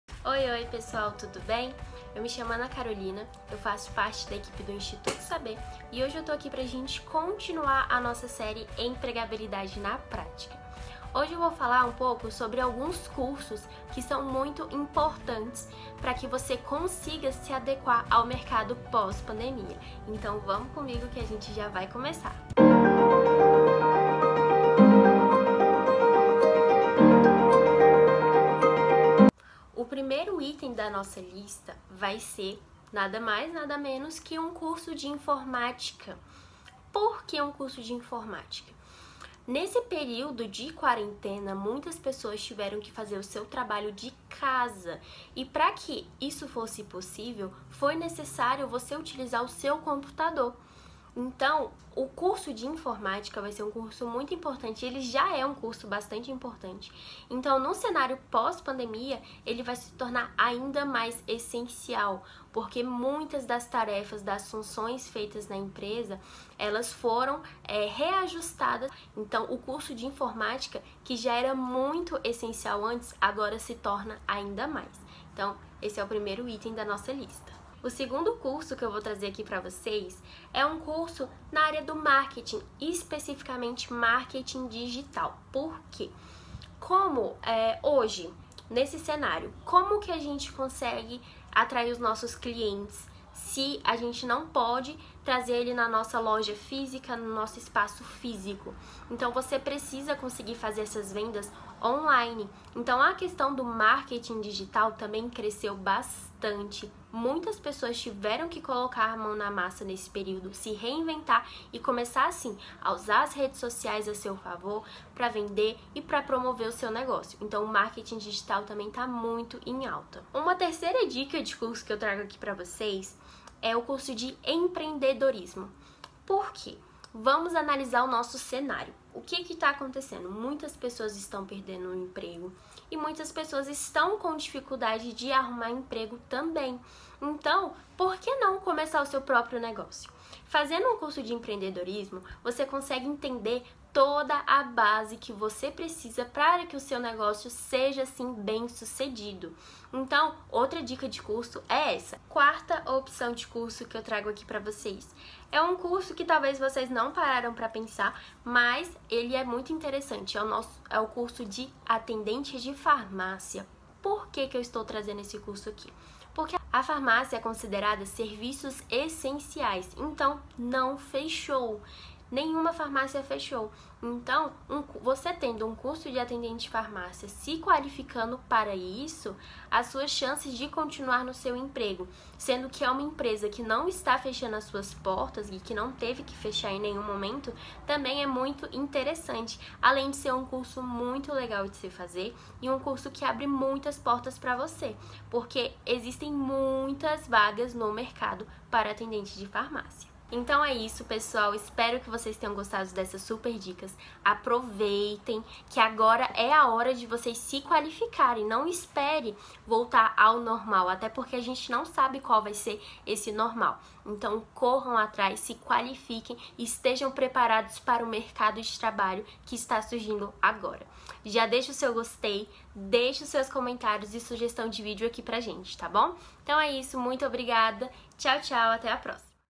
4 cursos para você investir no cenário atual
Nesse vídeo trazemos 4 cursos bem legais para você investir diante do cenário atual e que valem a pena!

Se interessou?? Entre em contato já